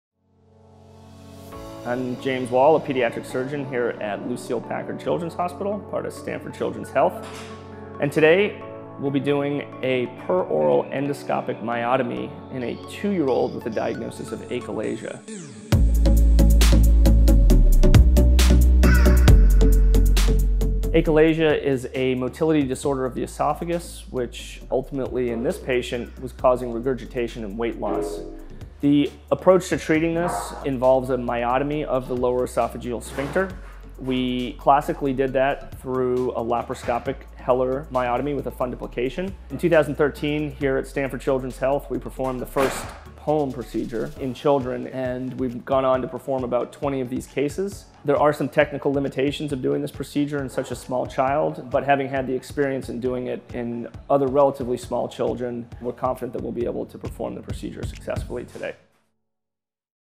Peroral Endoscopic Myotomy - POEM by Dr. James Wall | Case Trailer
The patient is a two-year-old with a diagnosis of achalasia. Achalasia is a motility disorder of the esophagus, which typically presents with poor, or disjointed peristalsis of the body of the esophagus, with a tight lower esophageal sphincter, that does not normally relax. This causes swallowing difficulties and ultimately in this patient was causing regurgitation and weight loss. It is an unusual diagnosis in someone this young, but has been described in other cases. The approach to treating this involves a myotomy of the lower esophageal sphincter. We classically did that through a laparoscopic Heller myotomy with a fundoplication. That was done through multiple small laparoscopic incisions in the abdomen. In 2013, here at Stanford Children's Health, we performed the first POEM procedure in children and we've gone on to perform about 20 of these cases, ranging in ages from, now two years old, all the way up to eighteen. There are some technical limitations in doing this procedure on such a small child, including not being able to use an overtube, and other instruments that we typically use. But having had the experience of doing it in other relatively small children, we're confident that we'll be able to perform the procedure successfully.